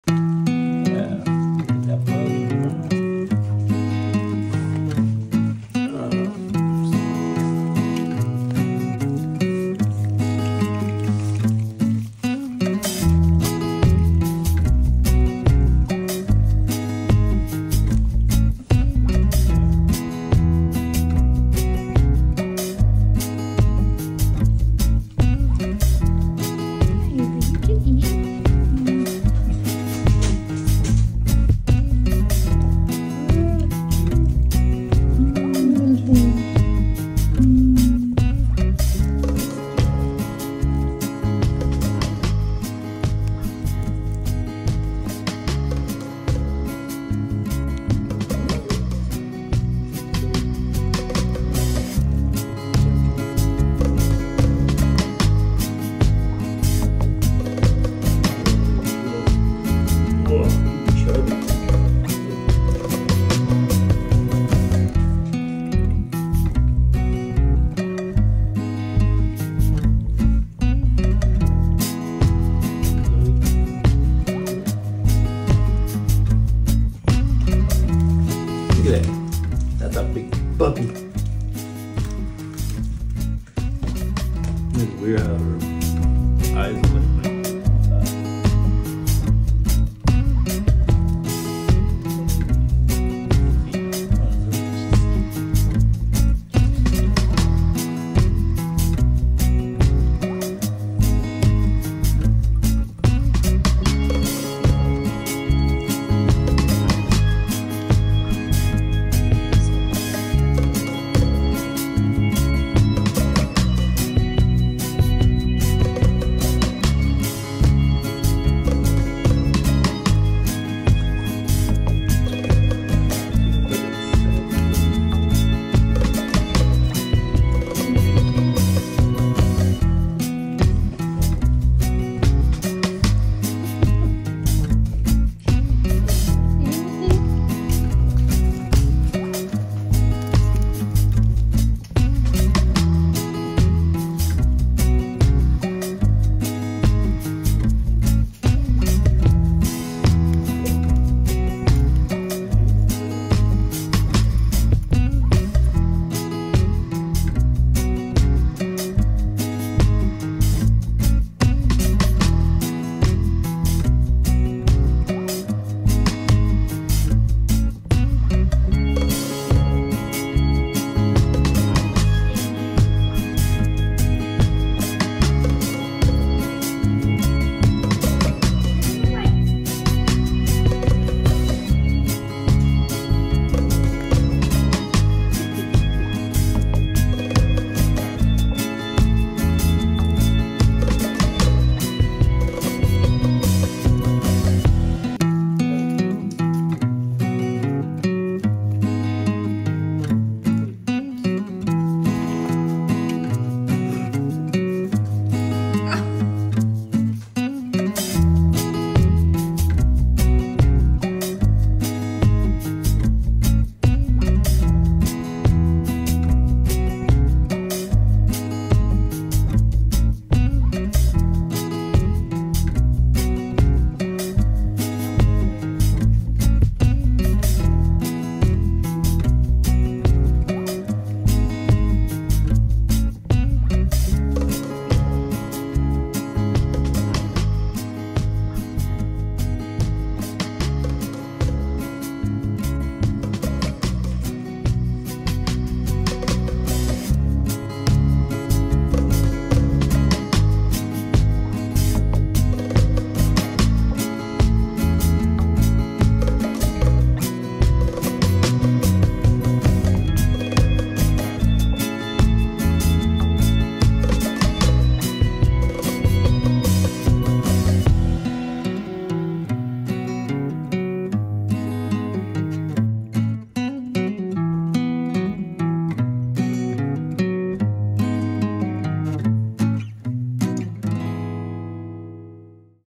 6 week old Chihuahua Puppies Growing Up - Feeding Time
6 week old Chihuahua Puppies Growing Up - Feeding Time